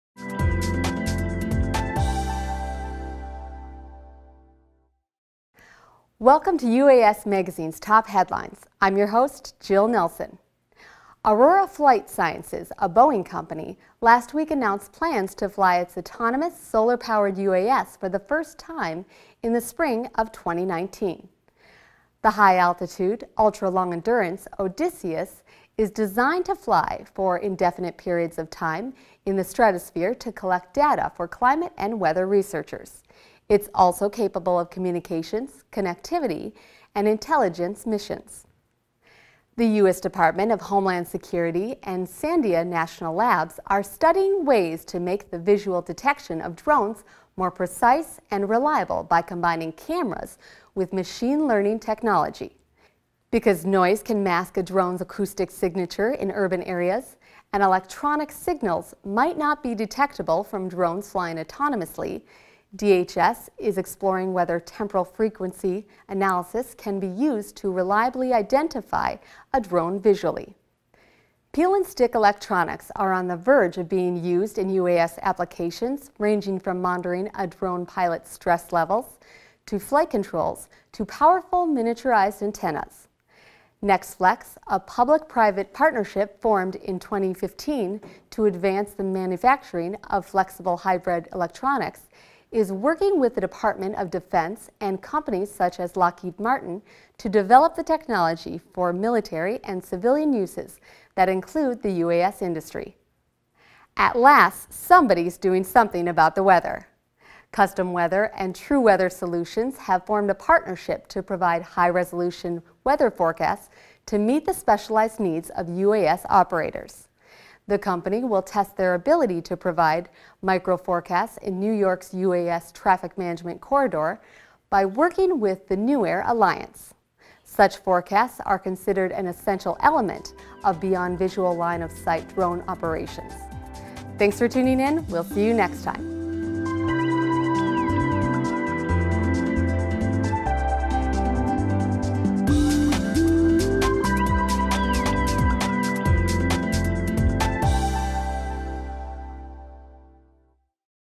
UAS Magazine's Top News - Week of 11.19.18
UAS Magazine’s Top News for the week of 11.19.18 covers the most critical information and in-depth storylines within the unmanned aerial system’s industry. Every two weeks, UAS Magazine’s Top News will highlight the most pressing issues about UAS policy, regulation, project finance and technology within the industry. UAS Magazine’s Top News is produced as a timely and short comprehensive video.